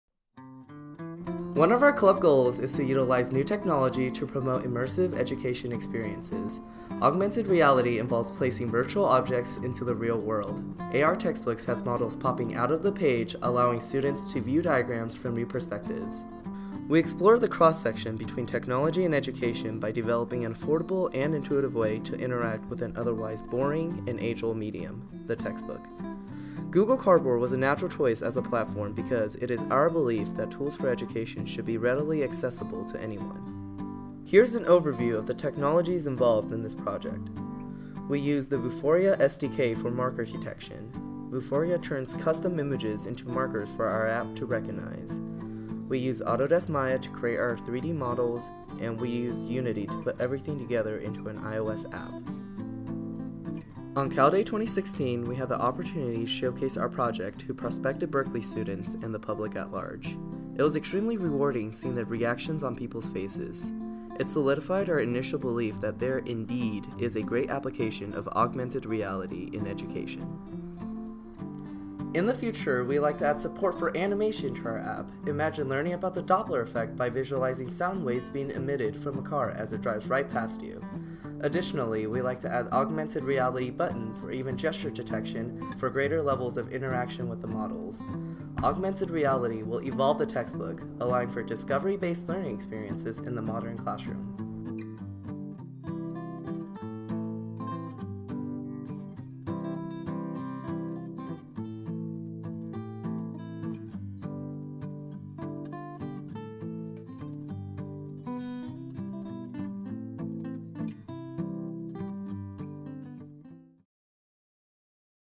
VR@Berkeley Augmented Reality Textbooks - Creating a new medium for learning
Ever wished you could take that diagram from your textbook and pull it out of the page to look at it from every angle? AR Textbooks allows you to use your phone to bring diagrams to life! By using a Google Cardboard, it allows full stereoscopic viewing of the diagram that virtually jumps out of the page.